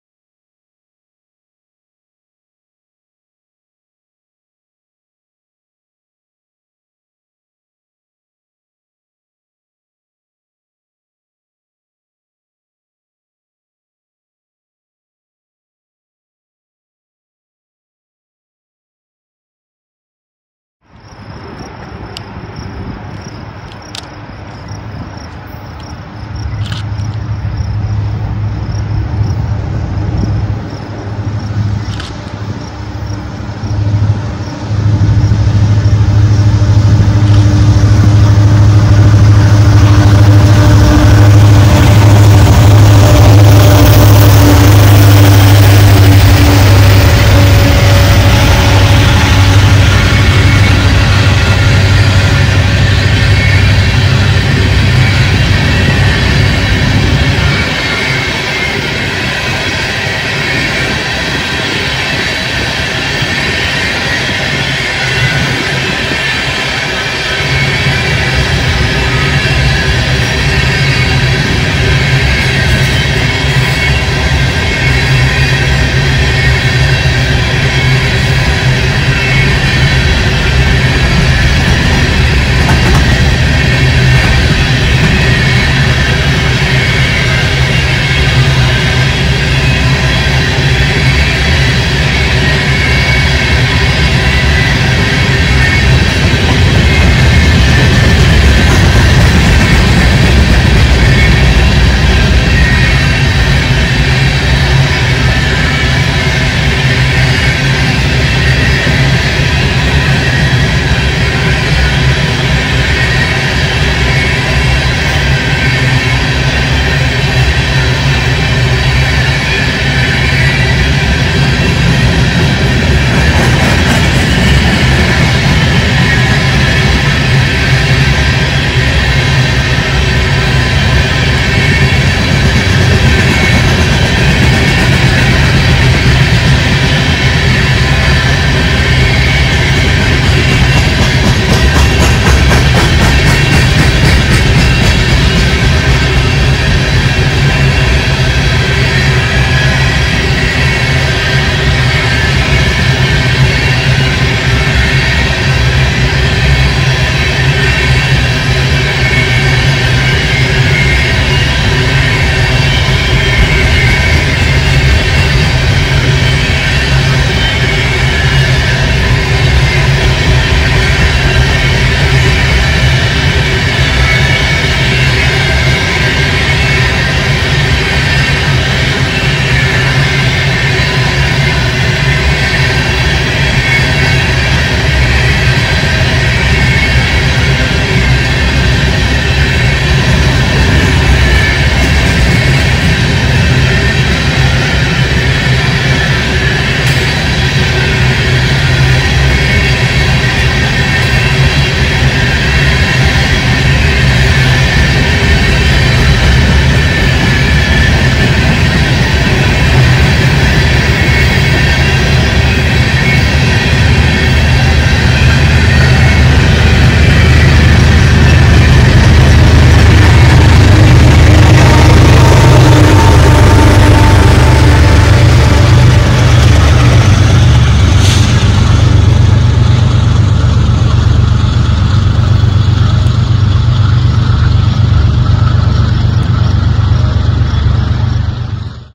5/19/24 BNSF SD70ACE 8775 & BNSF H2 AC4400CW NB bay coal with DPu BNSF ES44AC & BNSF H2 AC4400CW
Title: 5/19/24 BNSF SD70ACE 8775 & BNSF H2 AC4400CW NB bay coal with DPu BNSF ES44AC & BNSF H2 AC4400CW

Direction: North
Fright: Bay coal
Leader number: 8775
Locomotive leader: SD70ACE
Date: 5/19/24
Time: N/A
Mile post: 70
Horn: no horn
Signal: Clear
HeritageUnits power: H2
DPu/EOT: DPu
Train symbol: N/A

Leader

Bay coal